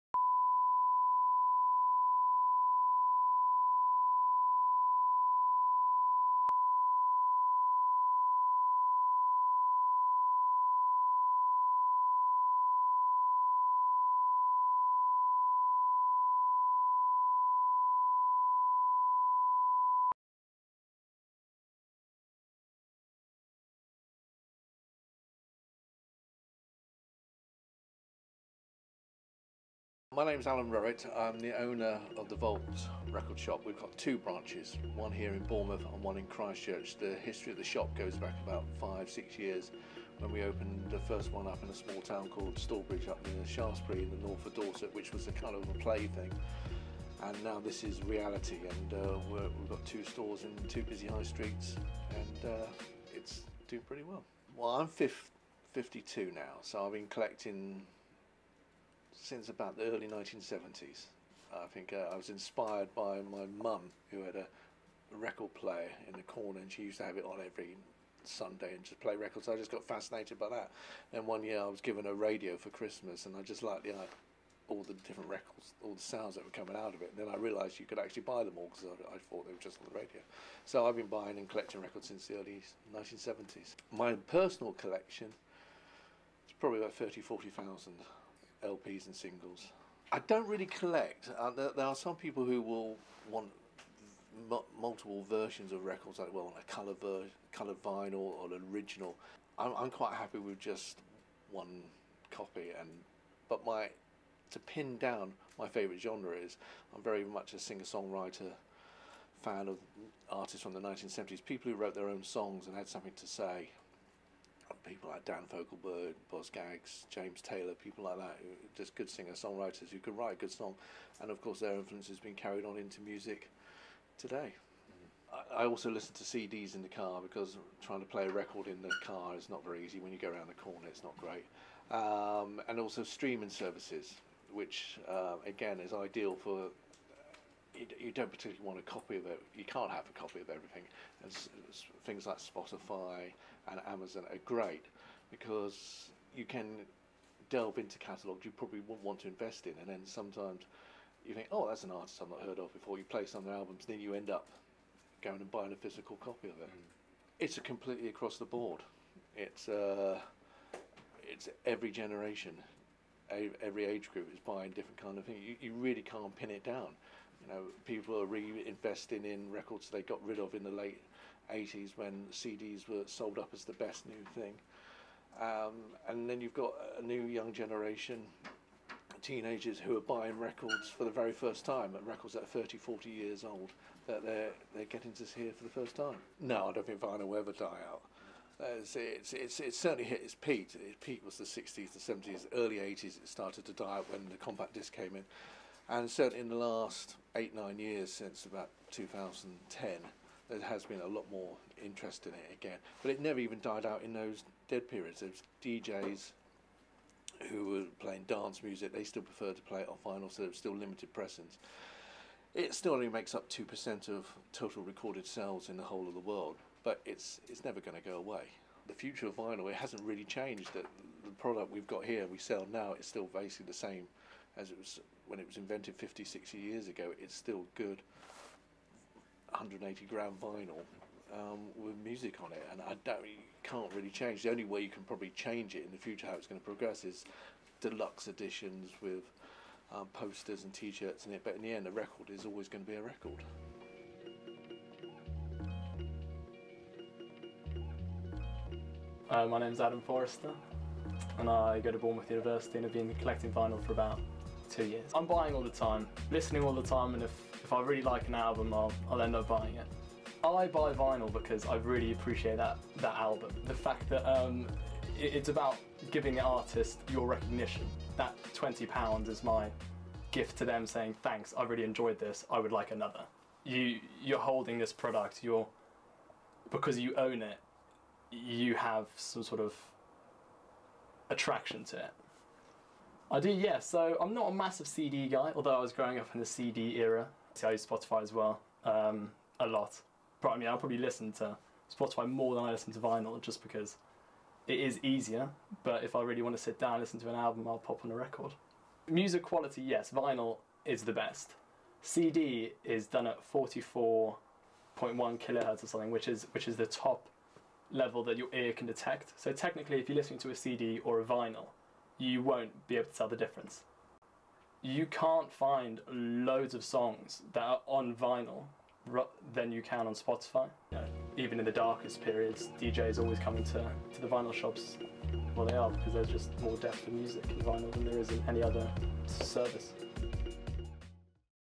"The records keep on spinning" Vinyl Documentary
BA (hons) Media Production L4 - Digital Film Unit "Diversity" Brief.

Producer and Camera Operator - Lizzie Beadle
Director- Freddie Betts
Audio Operator- Liam Bell
Editor- Alex Bessell